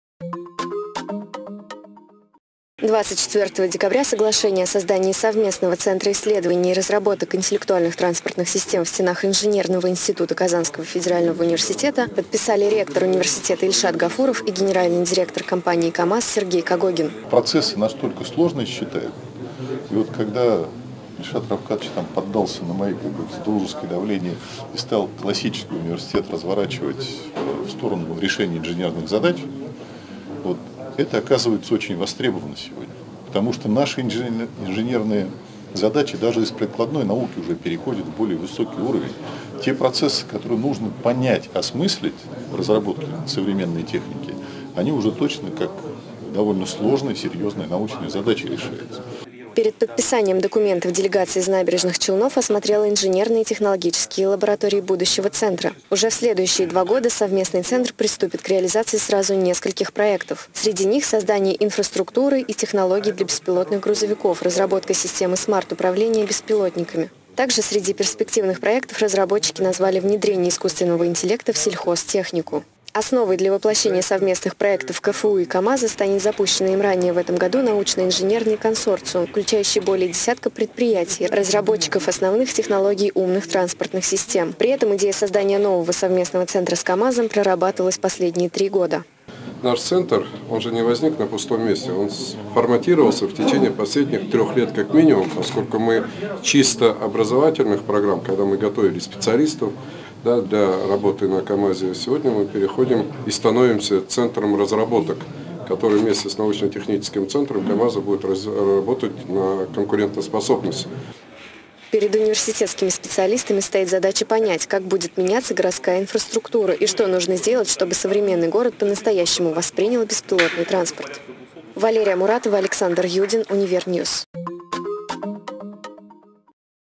Беспилотное транспортное средство
Казанский федеральный университет и ПАО «КАМАЗ» создают серию беспилотных транспортных средств на основе существующих и перспективных моделей автомобилей. Об открытии центра исследований и разработок интеллектуальных транспортных систем расскажет Валерия Муратова